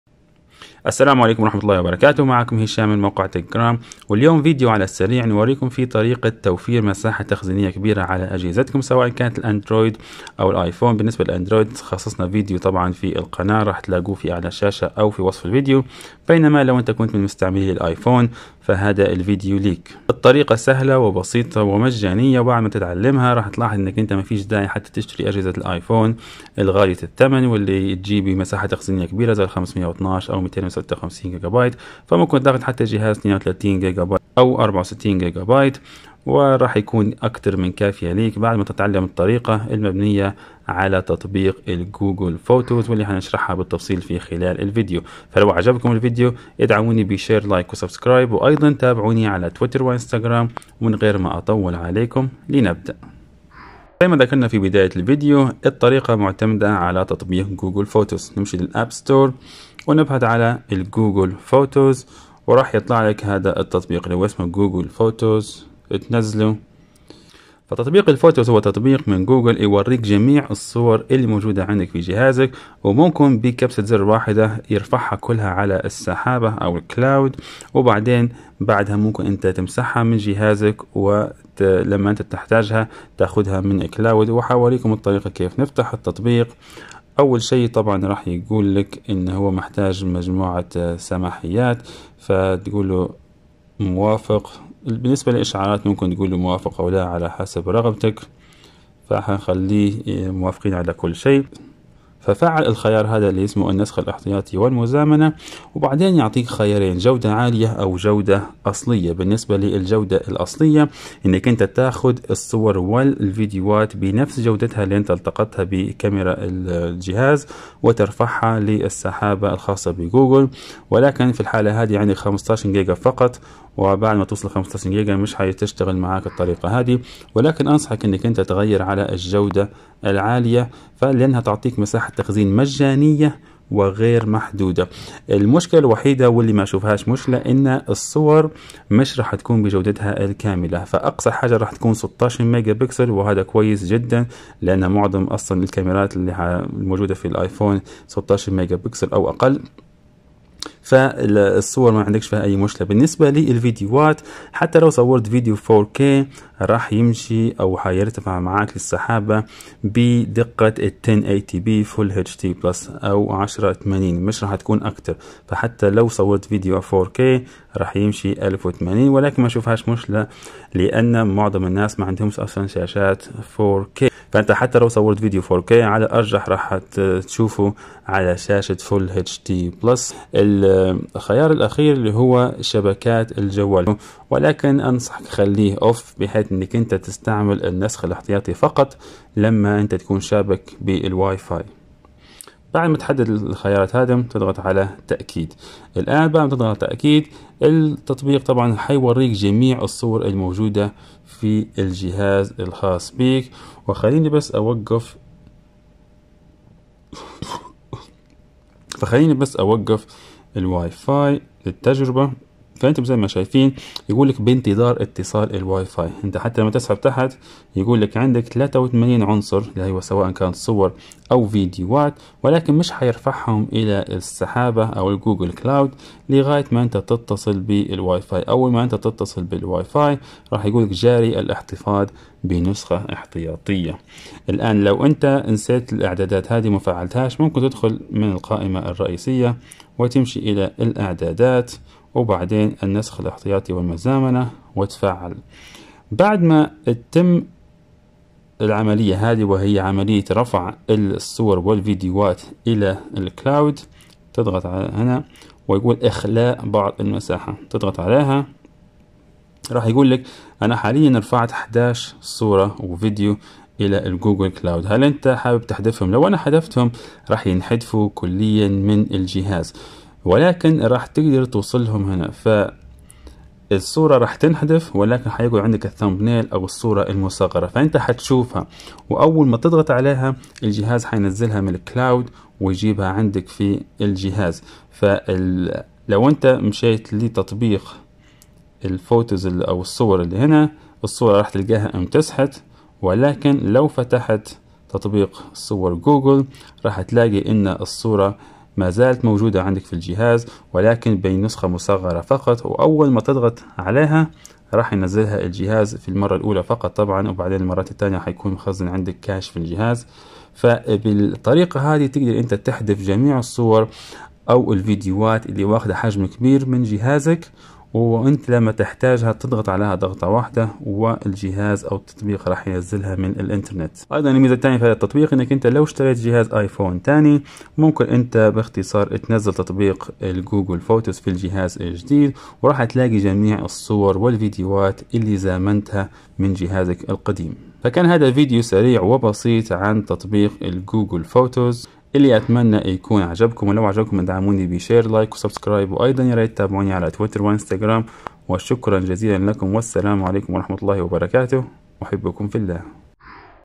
كيف توفر مساحة تخزينية في الايفون مجانا | حل مشكلة الذاكرة الممتلئة للايفون
مشكلة امتلاء الذاكرة الداخلية تعتبر أحد أكبر المشاكل التي تواجه المستخدمين خصوصا لمحبي التصوير وتسجيل الفيديوهات .. في هذا الفيديو سنتعرف على أفضل طريقة مجانية يمكن الاعتماد عليها لتوفير أكبر قدر من المساحة الداخلية في هاتفك الايفون